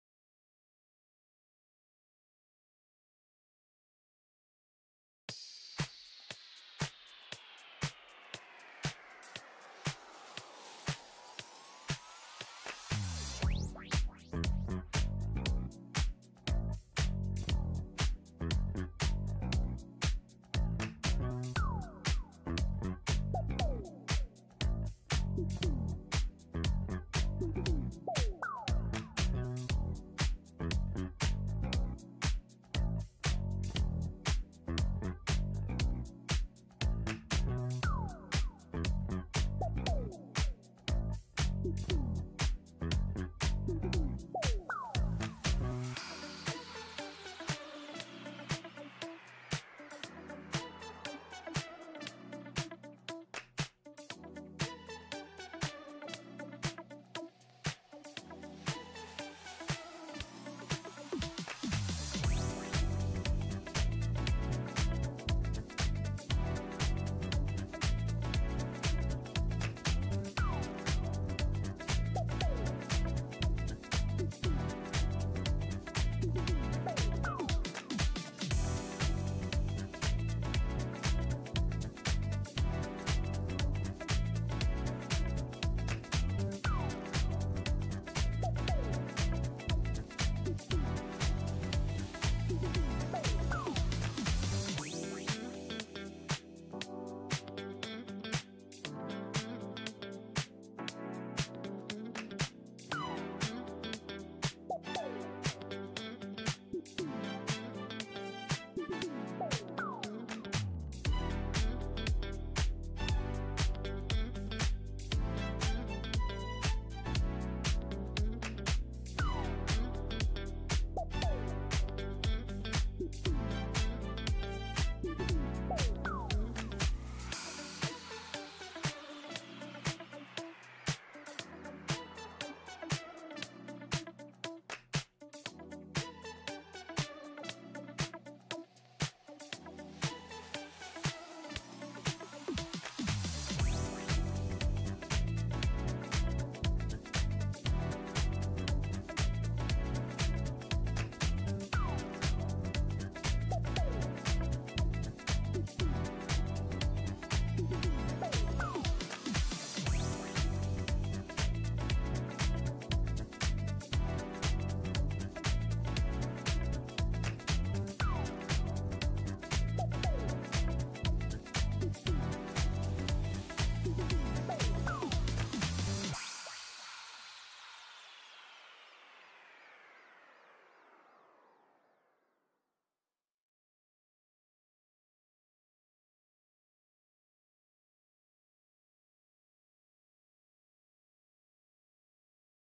How to draw Envy from inside out 2
Easy step by step drawing of envy from inside out 2